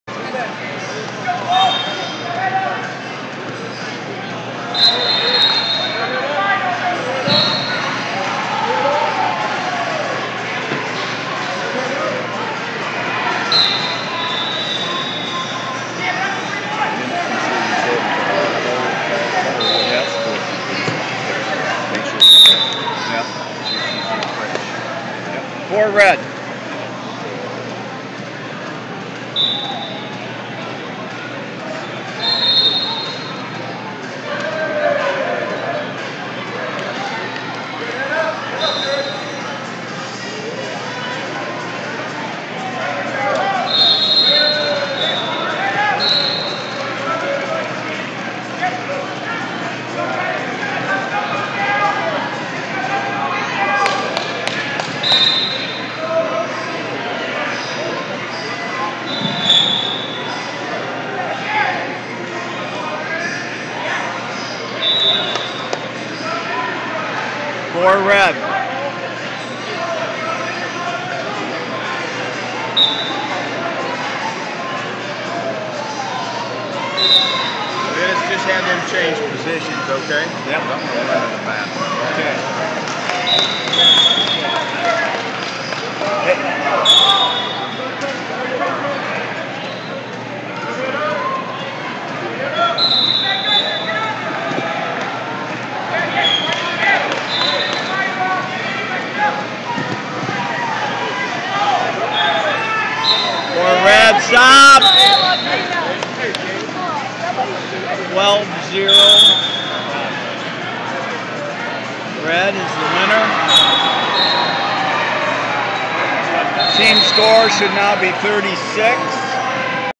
Mat 7 136 Paul Ognissanti Maryland Vs Riggin Boger Utah
2019 14U Schoolboy Duals - Mat 7 136 Paul Ognissanti Maryland Vs Riggin Boger Utah